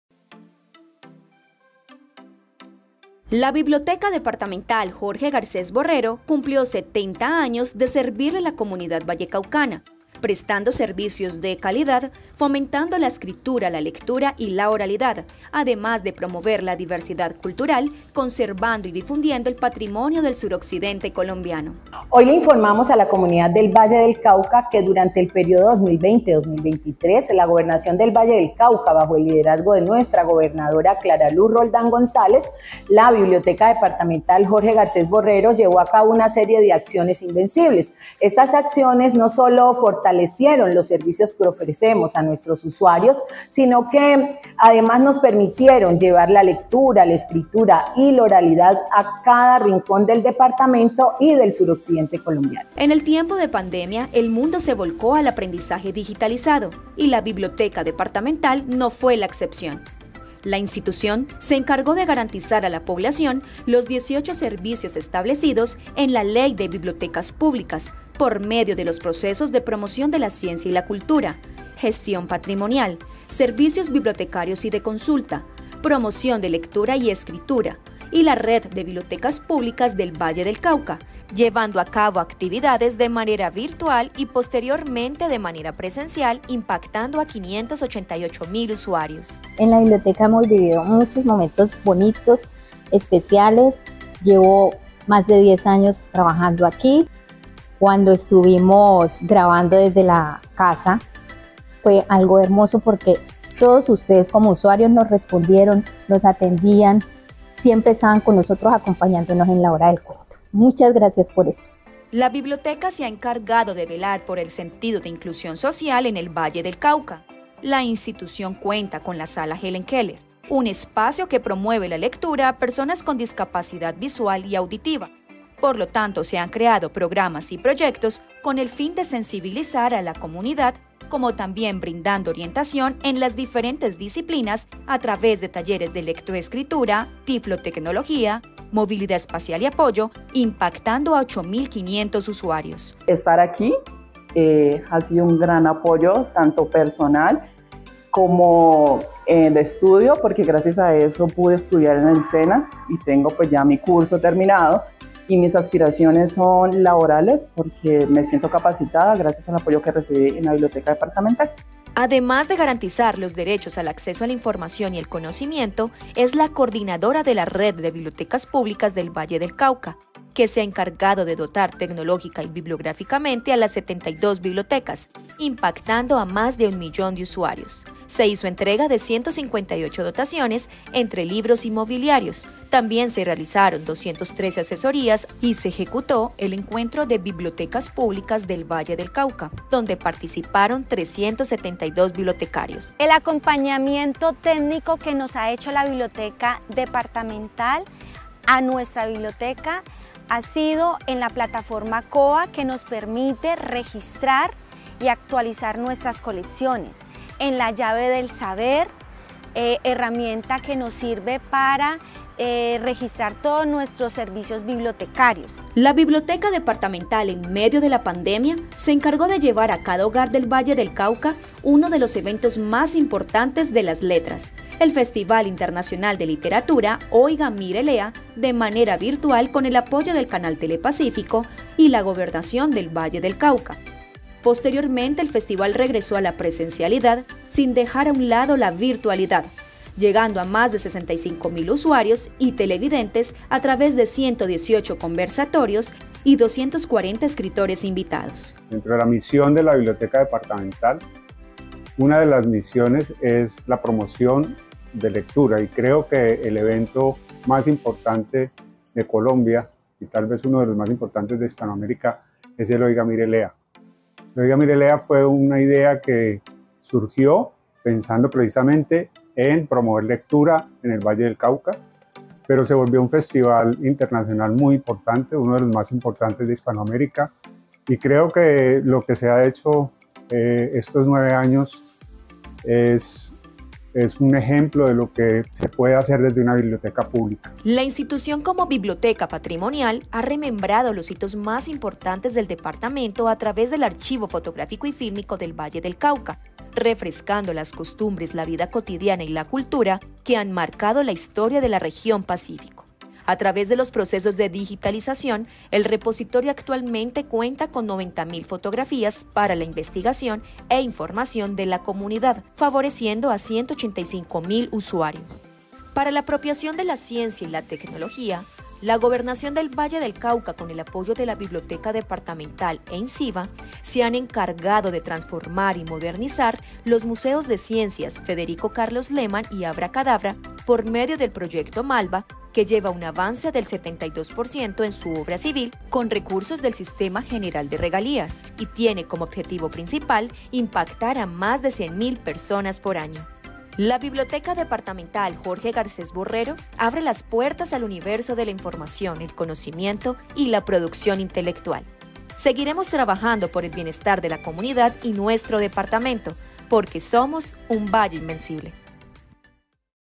GESTIÓN INSTITUCIONAL 2020 - 2023 DEL GOBIERNO VALLE INVENCIBLE: BIBLIOTECA DEPARTAMENTAL
✔Gestión Institucional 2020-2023, Biblioteca Departamental Jorge Garcés Borrero

Logros alcanzados durante los 4 años de gobierno de nuestro valleinvencible💪